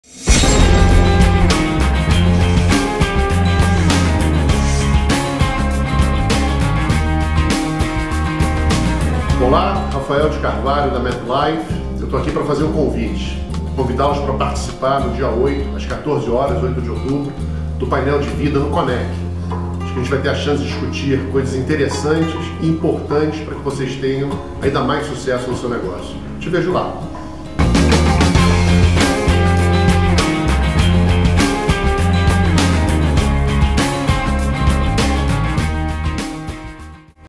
MetLife estará em painel sobre seguro de vida
O presidente da MetLife, Raphael de Carvalho, convida os corretores de seguros para o 17º Conec.